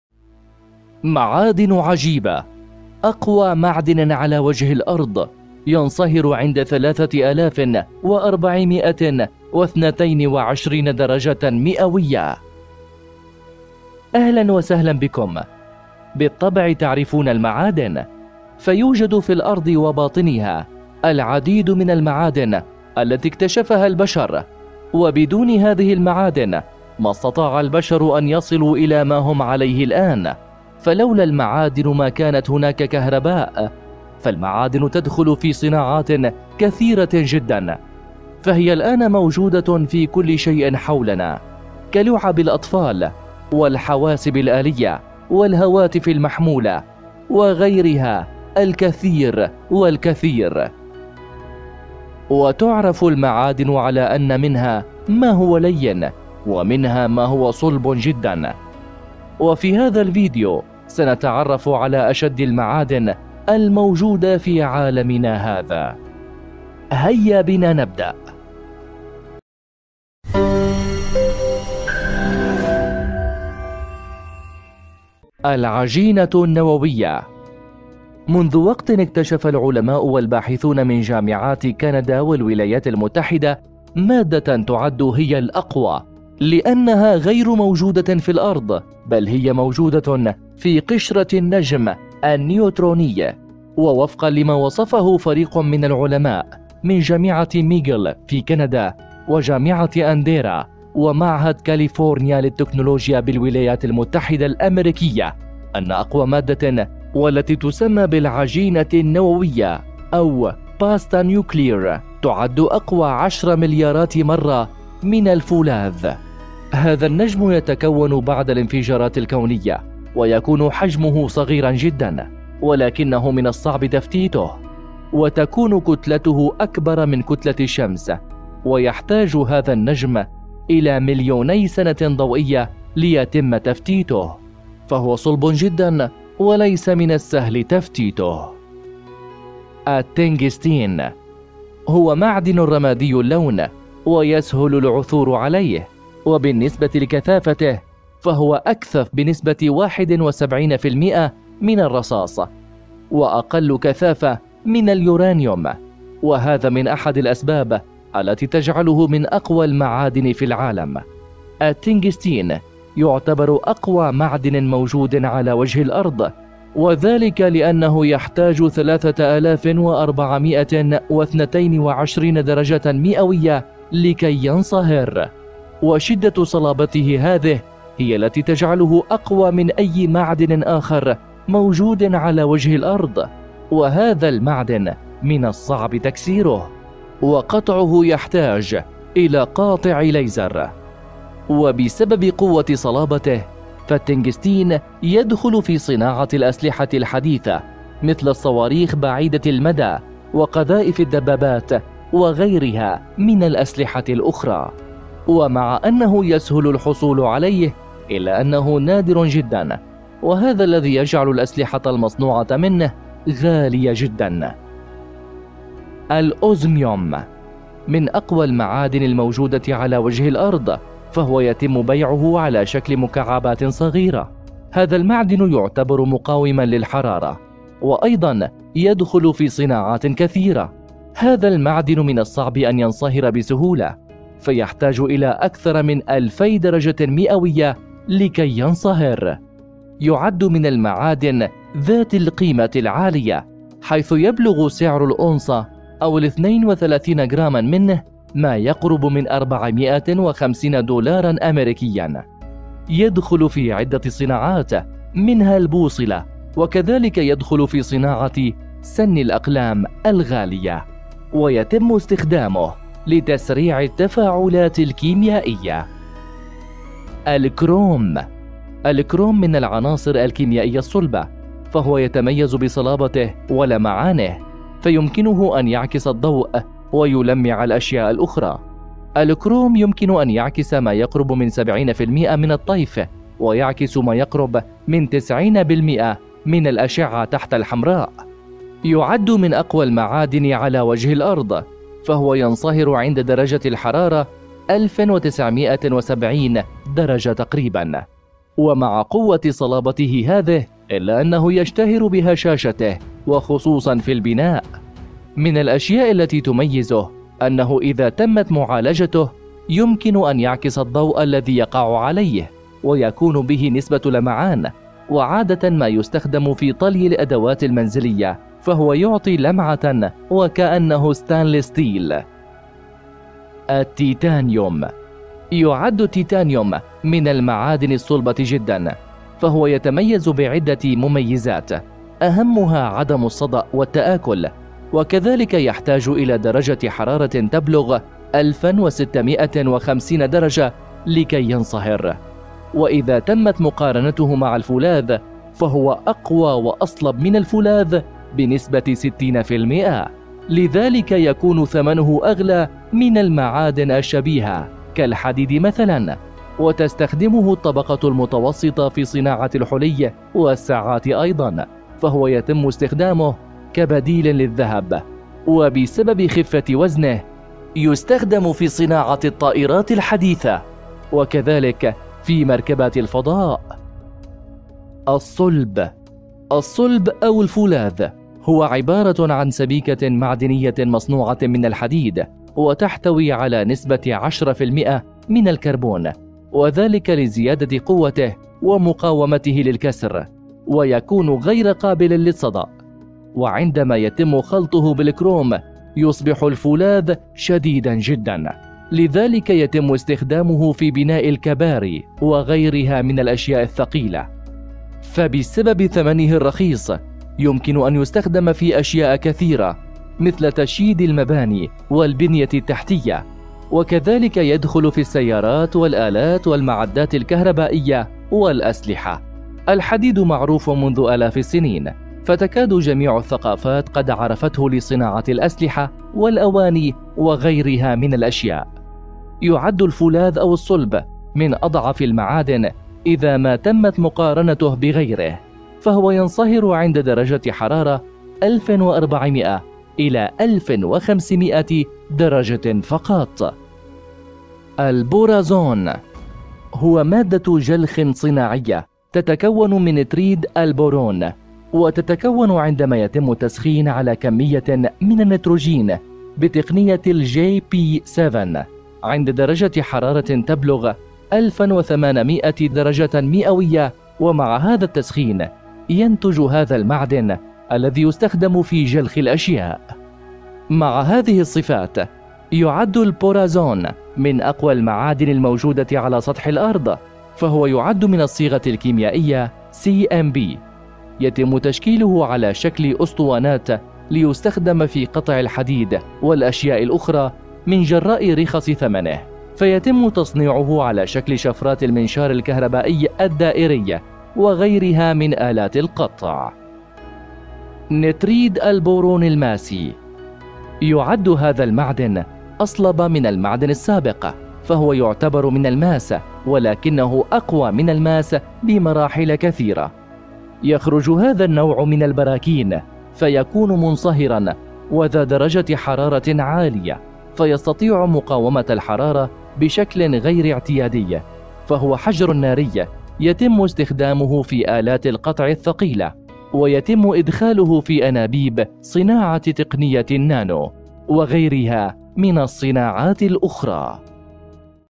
أقوى من الفولاذ وأخف من الهواء ! أقوى معدن على وجه الأرض .. ينصهر عند 3422°C درجة مئوية !
بالطبع تعرفون المعادن ، فيوجد في الأرض وباطنها العديد من المعادن التي أكتشفها البشر ، وبدون هذه
 المعادن ما استطاع البشر أن يصلوا إلى ما هم عليه الآن ، فلولا المعادن ما كانت هناك كهرباء ، فالمعادن تدخل في صناعات كثيرة جداً ، فهي الآن موجودة في كل شيء حولنا ، كلعب الأطفال والحواسب الألية والهواتف المحمولة وغيرها الكثير والكثير ..

وتعرف المعادن على أن منها ما هو لين ، ومنها ما هو صلب جدا ، وفي هذا الفيديو سنتعرف على أشد المعادن الموجودة في عالمنا هذا ..

هيا بنا نبدأ

للمزيد من المقالات والفيديوهات الرائعة

جميع الحقوق محفوظة لدى قناة غذي ذهنك

♥️ اِسْتَغْفَرَكَ رِبِّيُّ مِنْ جَمِيعَ الذُّنُوبِ وَالْخَطَايَا واتوب الِيكَ ♥️